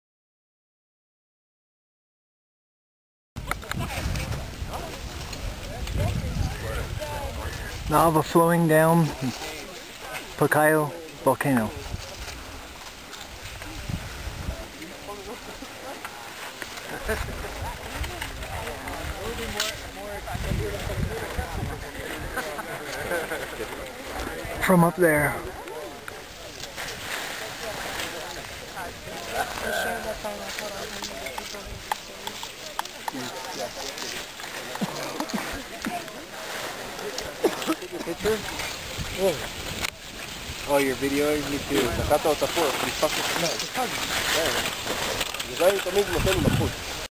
Guatemala, Pacaya Volcano
Guatemala, Pacaya Volcano -- standing within 10 feet of the flow of lava at sunset